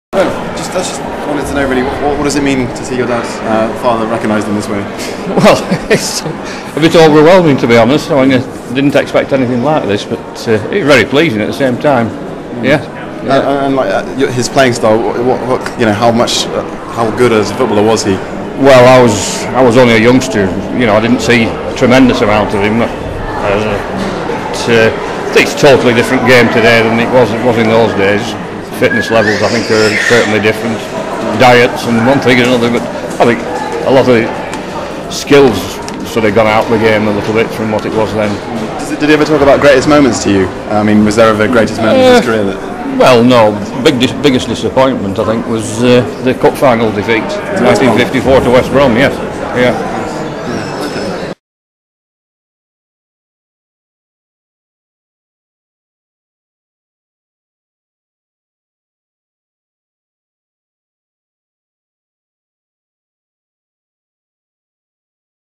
Brian Finney interview
On Sir Tom Finney Day The Score's Jamie Renton got a chance to speak to the former Preston North End great's son Brian